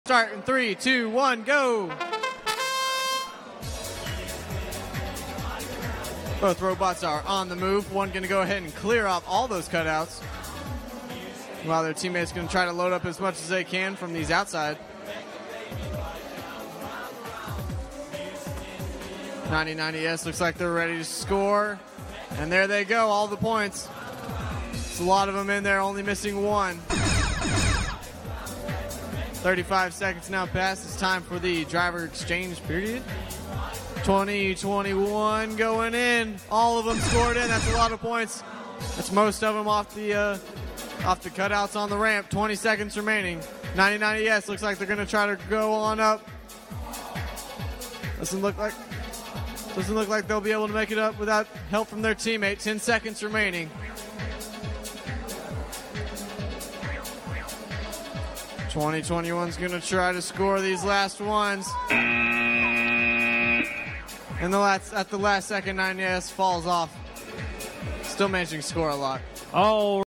VEX Worlds 2016 - VEXIQ Middle School - Math - Qual 302 (2021 9090S) 110
VEX Worlds 2016 - VEXIQ Middle School - Math - Qual 302

Team Score 110:
2021 - Dana Point Robo-Techs (Dana Point Robotics Club, Laguna Niguel, California, United States)

9090S - T-VEX (The Mandarin Chinese School, Arlington, Texas, United States)